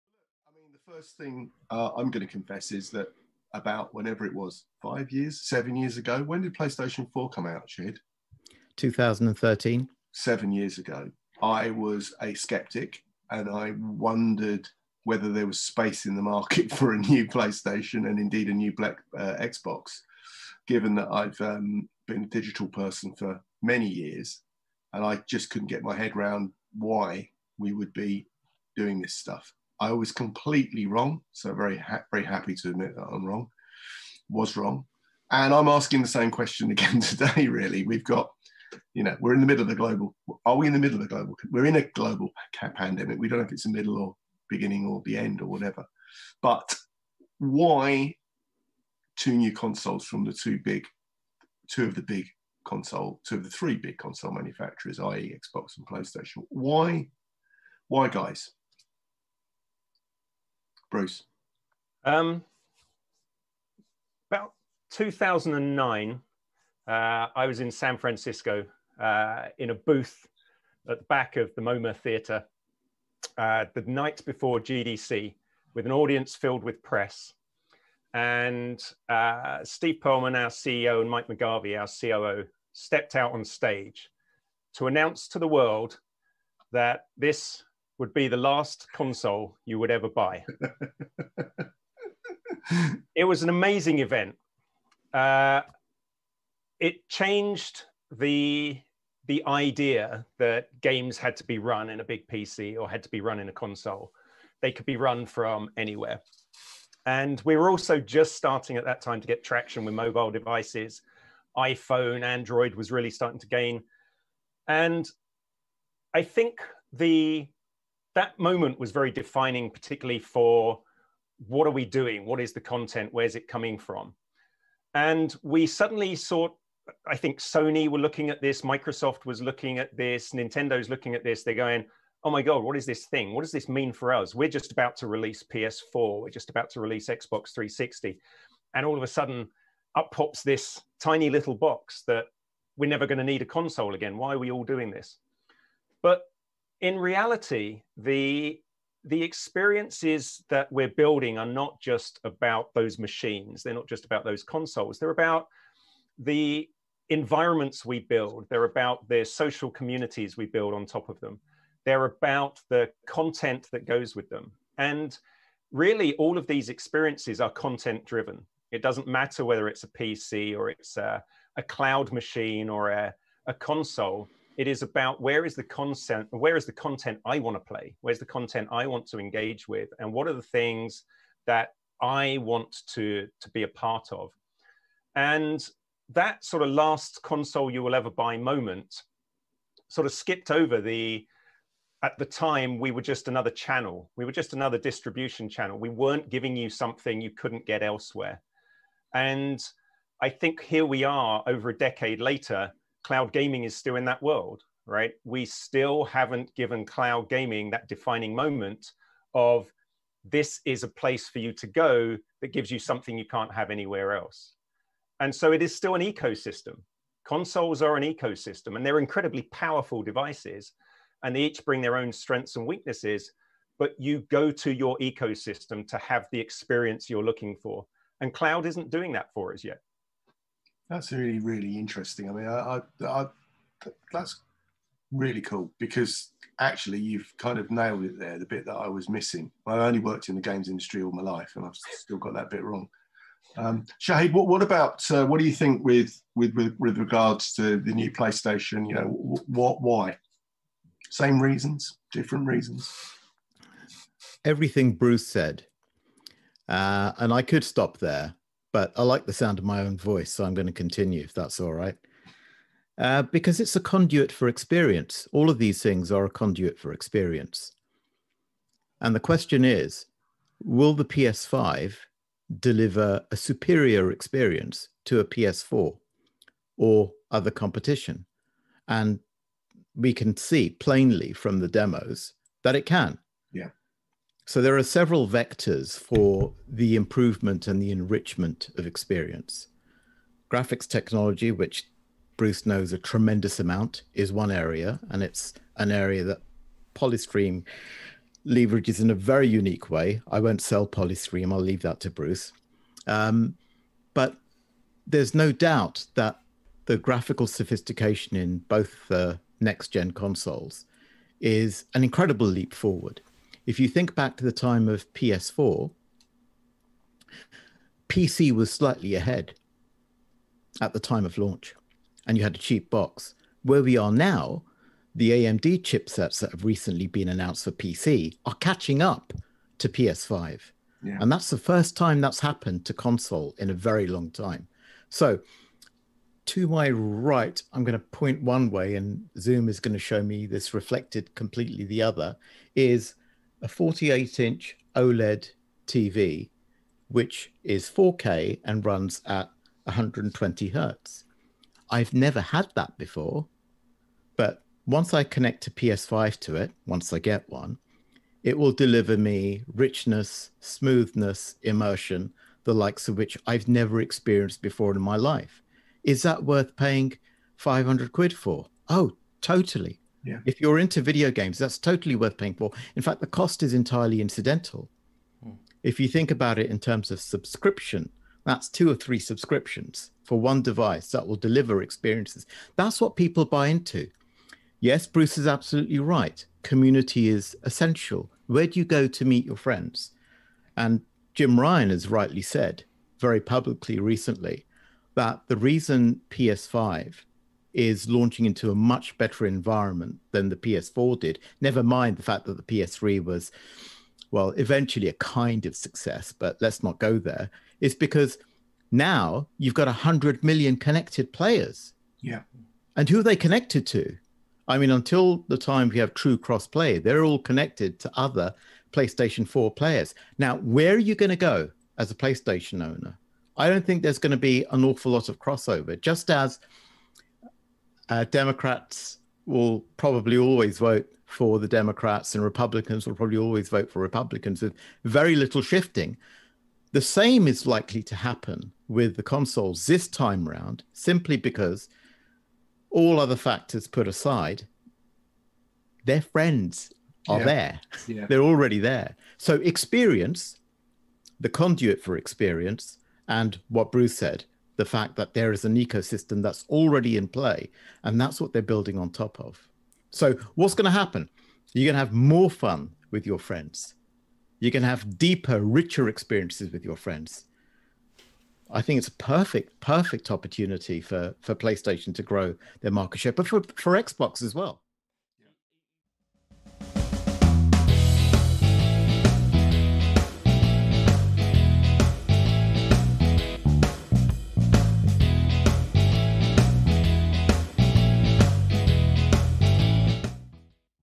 The Next Generation: The Timing of Console Releases
With a new generation of consoles just around the corner, Andy and Spike ask Bruce Grove and Shahid Kamal Ahmad about the timing of this year's first steps into the new generation.